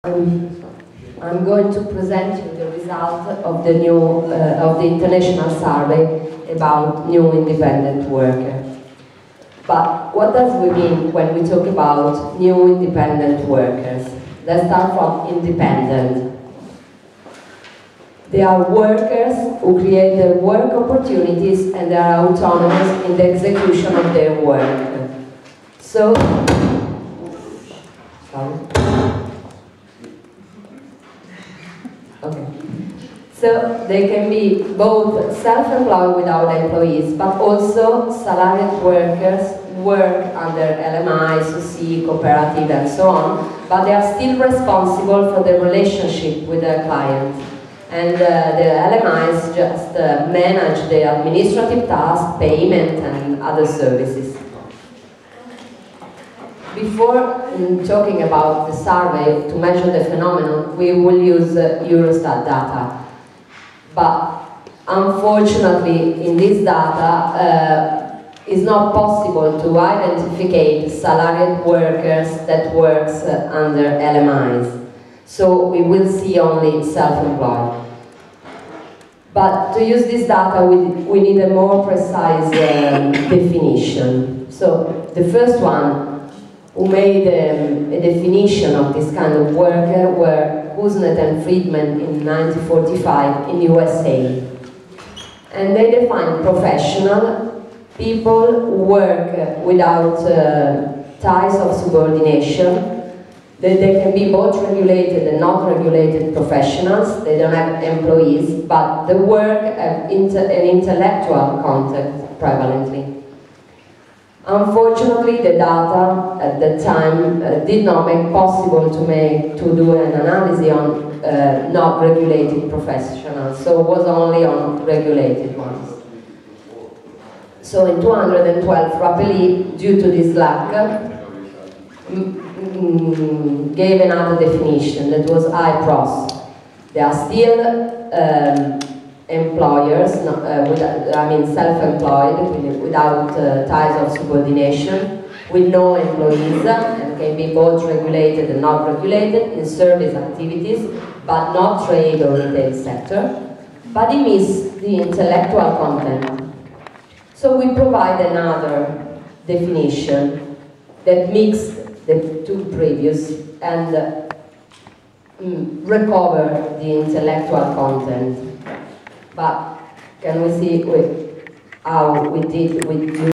SMart - iWire final conference - 16-03-2018 - video 3
Needs and socio-economic conditions of freelancers: results of the survey, presentation by Cristina Zanni (founder of ACTA: Associazione di Consulenti nel Terziario Avanzato).

More info about the iWire study "independent workers and the industrial relations in Europe"